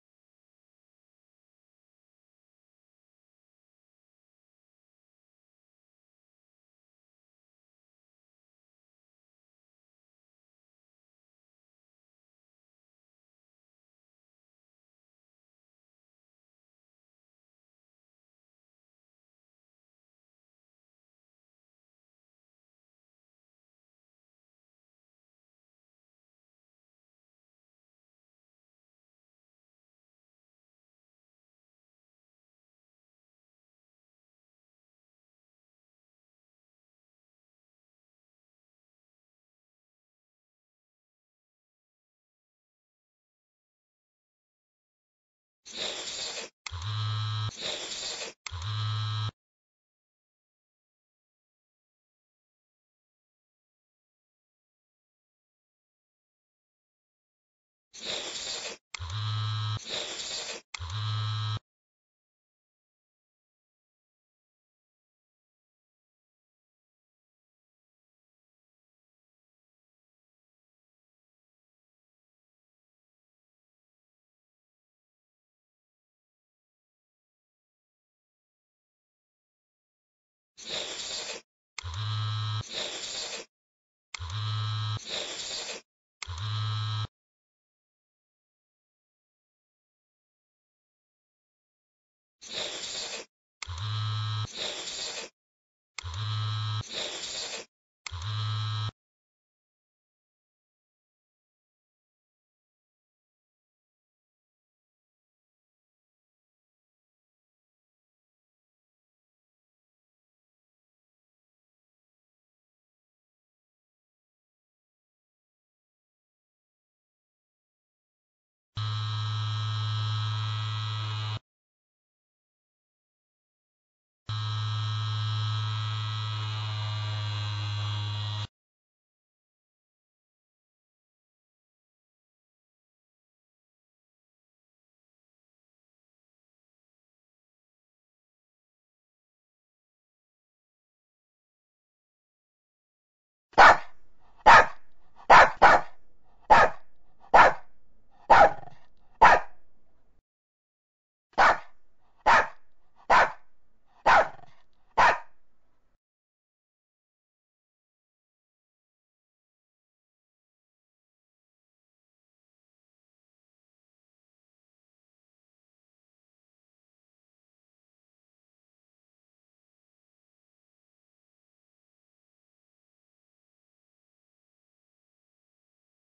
Ring tin tin re-edited
Ring tin tin is a social software that gives life to your phone. The devices search for each other and push you to meet familiar strangers in public places